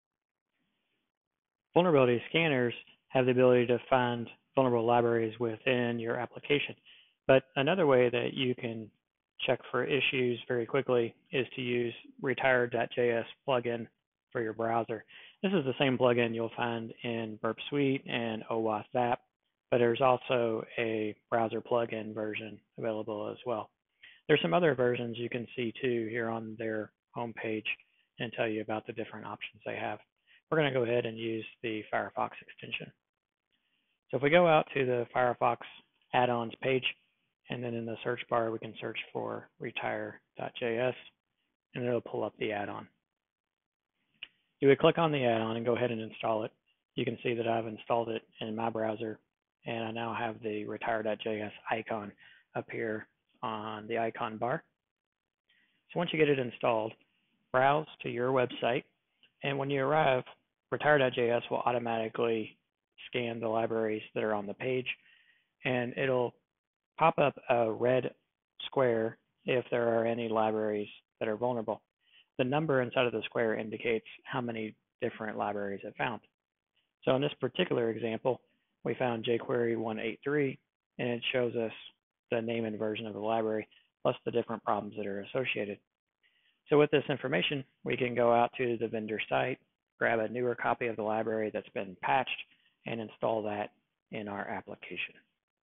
Check for Vulnerable Libraries in Your Web Application
Open-source JavaScript libraries receive frequent patches; some of which are to patch security vulnerabilities. However, application developers might not keep track of available updates unless monitoring the libraries is easy. The publicly sourced JavaScript libraries can be checked for security vulnerabilities right from the browser. OWASP Mutillidae II is a free web application security testing environment that can be used to learn more.